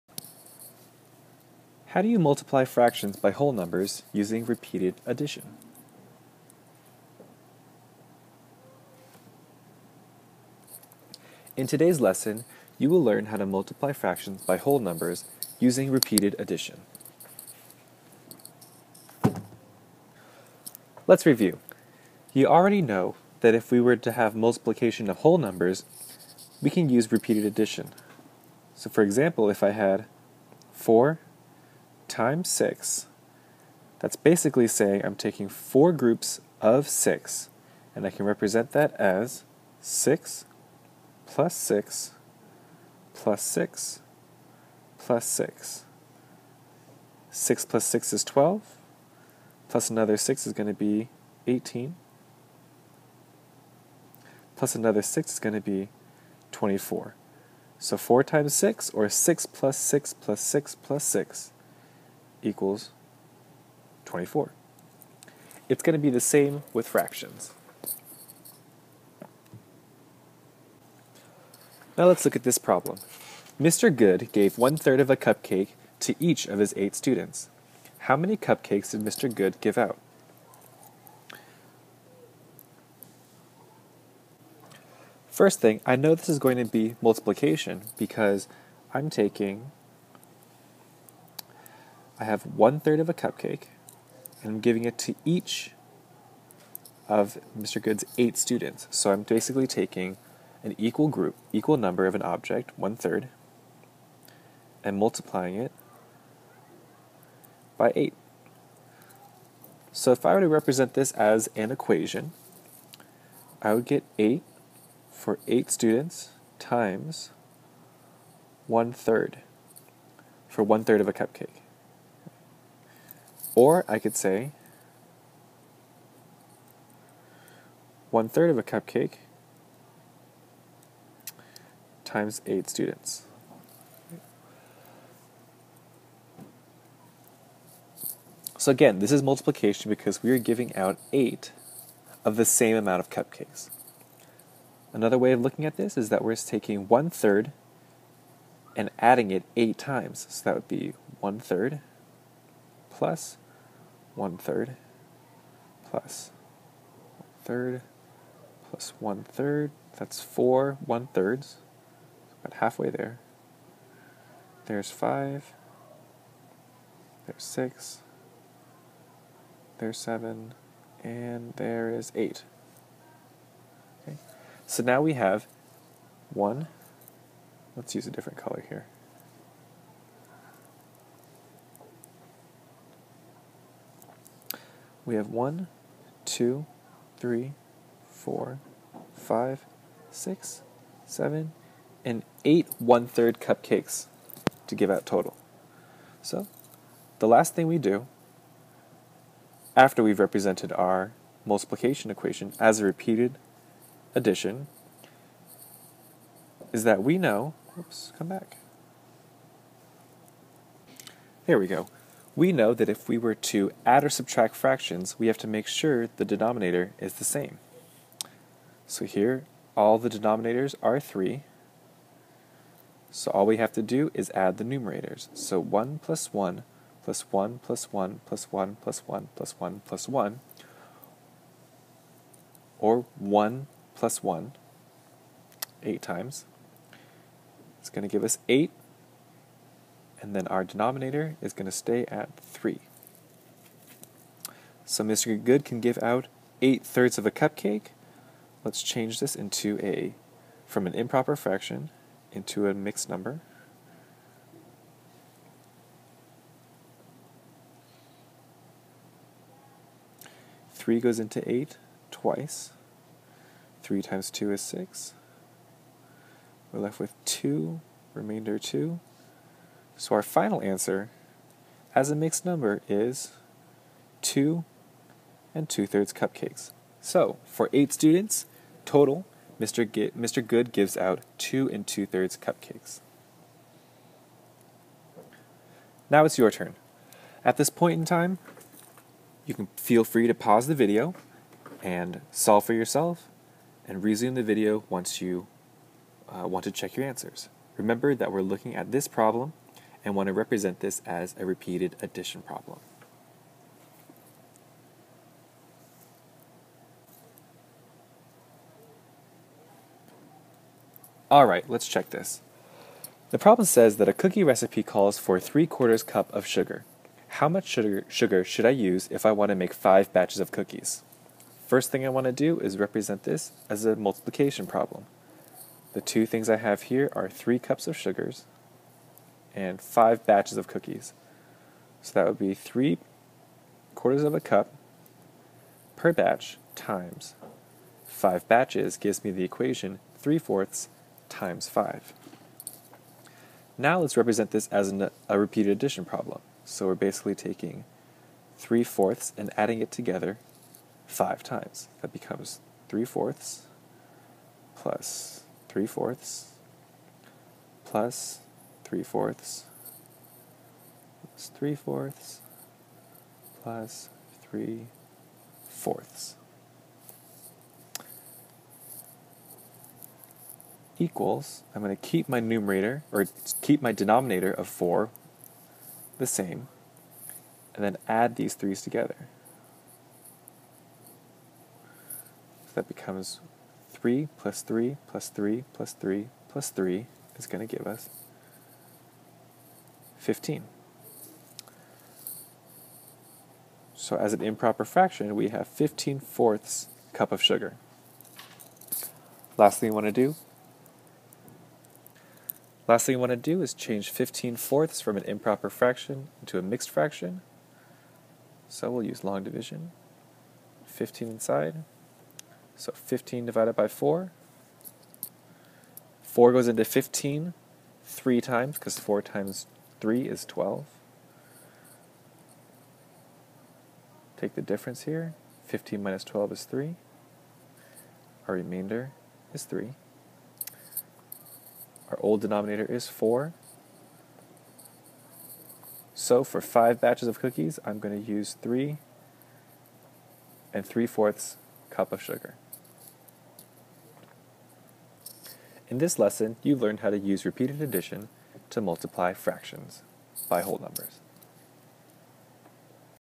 Multiplying Fractions By Whole Numbers
This lesson will teach you how to multiply fractions by whole numbers. Aligned with CCSS 5.NF.4.a.

Created for Mr. Pham's 5th grade class.